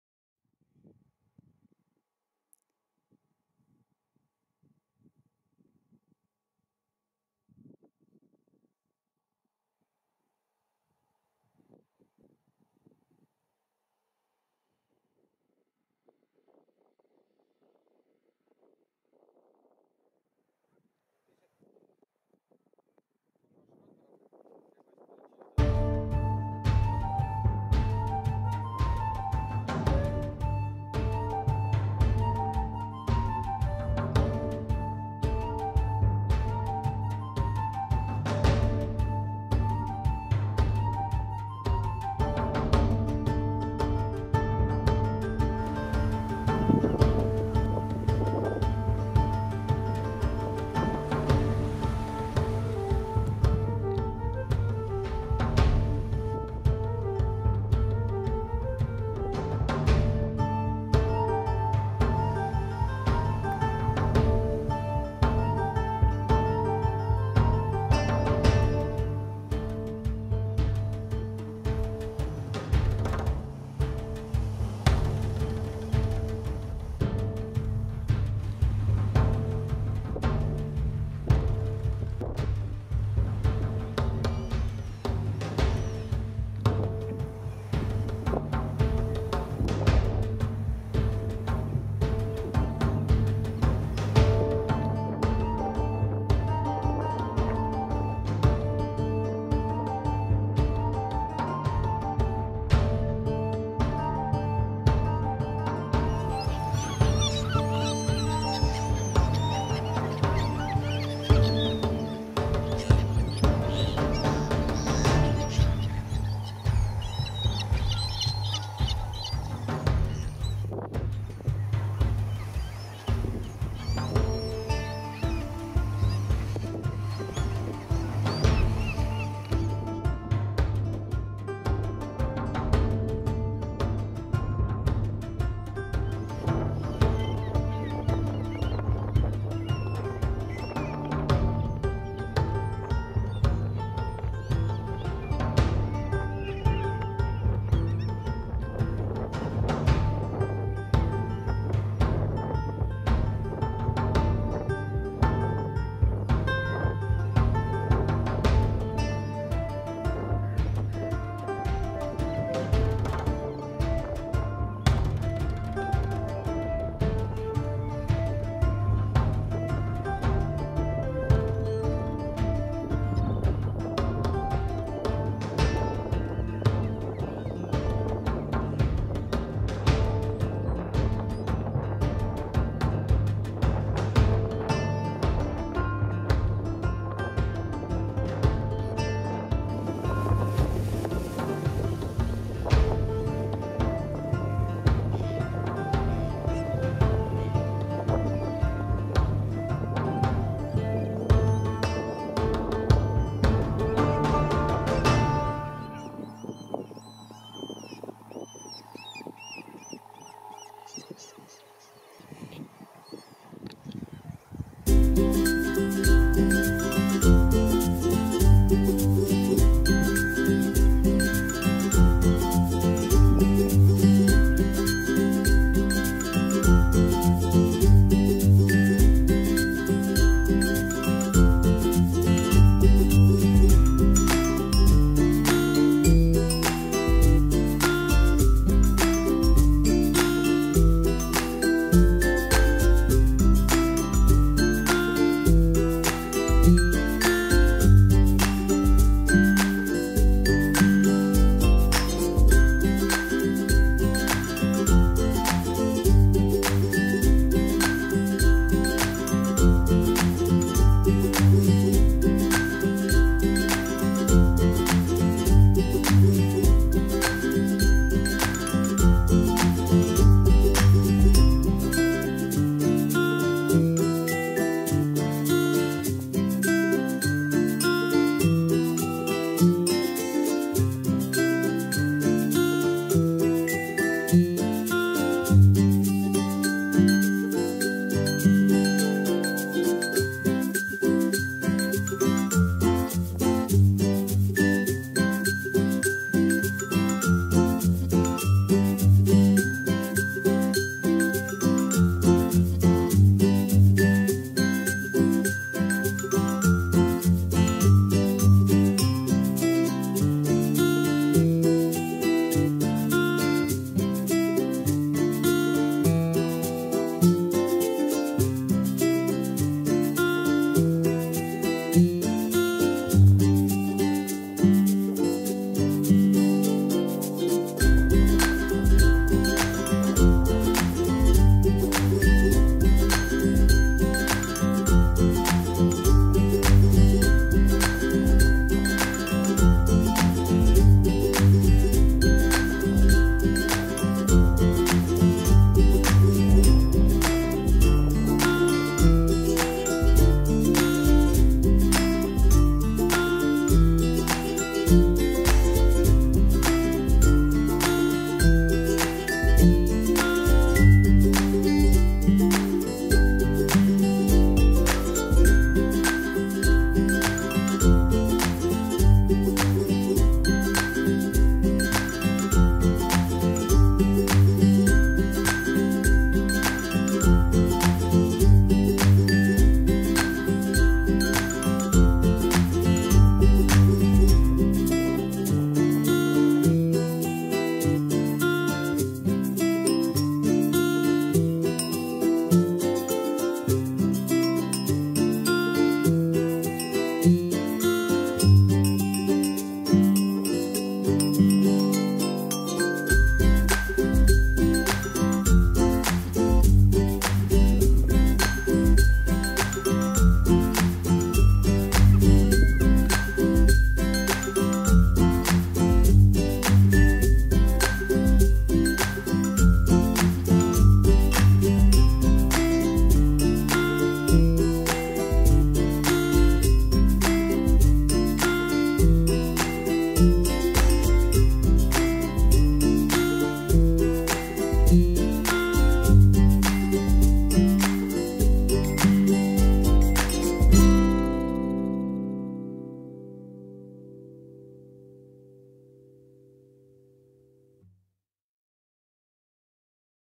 2022-09-10 Ellie's Walk - Late Summer in Strandhill
Ellie the Irish Softcoated Wheaten Terrier takes one of her usual walks at the Strandhill coast in the late summer sunshine.